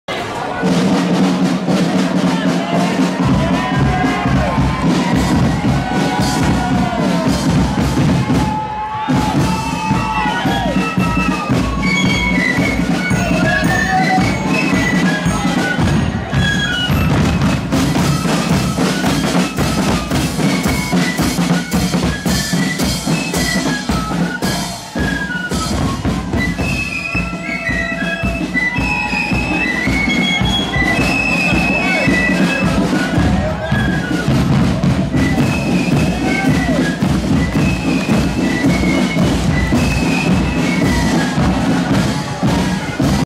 Regimental F.B. (No.2) @ Belfast 12th ~ Return Route ~ 12/07/22 (4K)
Regimental Flute Band parading down the Lisburn Road at the annual Belfast 12th parade (Return Route) held on Tuesday 12/07/22.